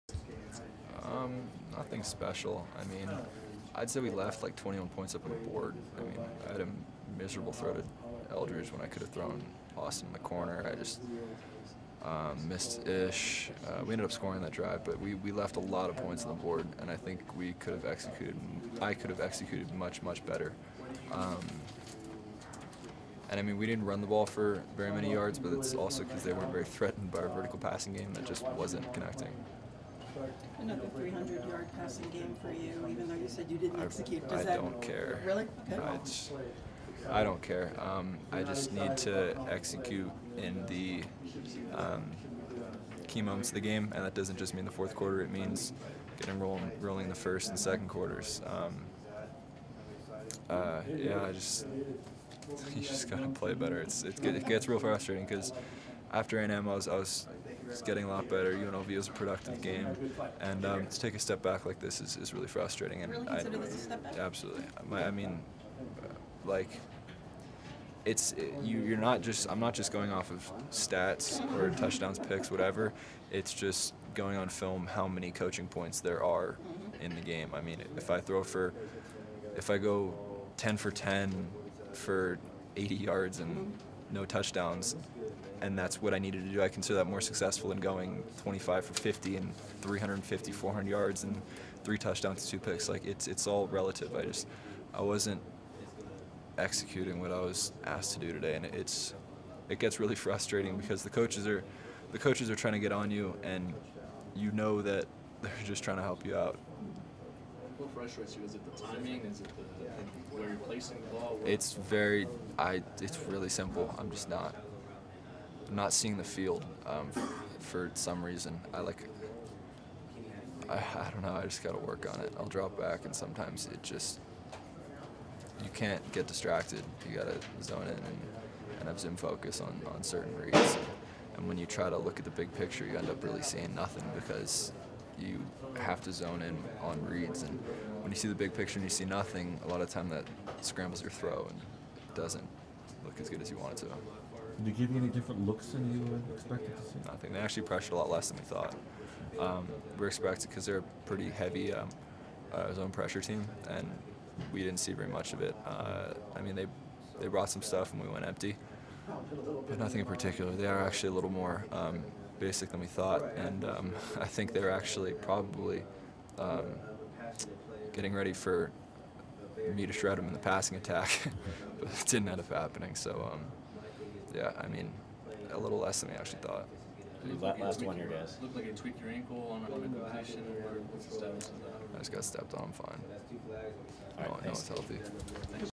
Josh Rosen Post Game Presser - Sept. 17, 2016
Despite completing 26 of 40 for 307 yards and two TD's, UCLA quarterback Josh Rosen wasn't happy with his performance in the Bruins' 17-14 victory over BYU in Provo Sept. 17, 2016